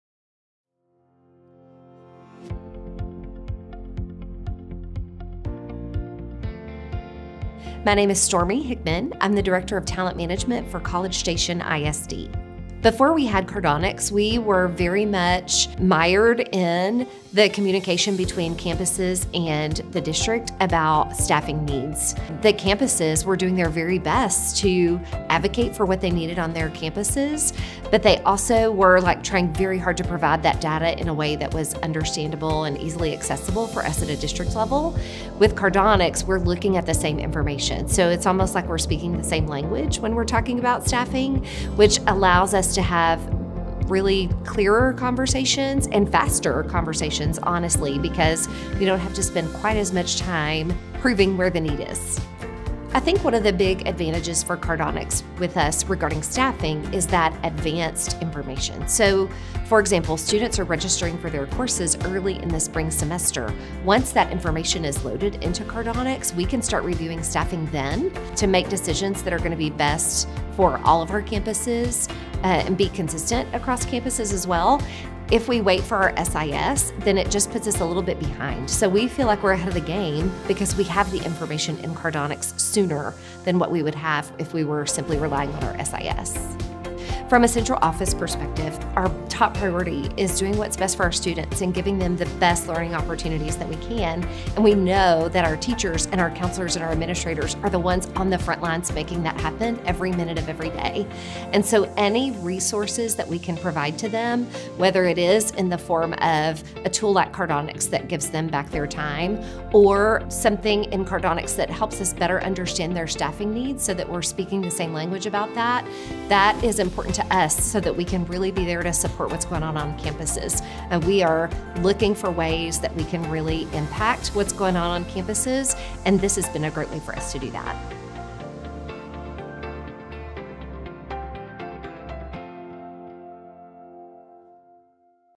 Building a Staffing Plan in Record Time
Stormy Hickman, Director of Talent Management for College Station ISD shares her experience using Cardonex® Staffing and Master Scheduling.

"With Cardonex, we are looking a the same information ... we are speaking the same language when we are talking about staffing... you don't have to spend quite as much time proving where the need is. "

"One of the big advantages is the advanced information. Once that information is loaded in Cardonex, we can start staffing then. If we wait for our SIS, it puts us a little bit behind."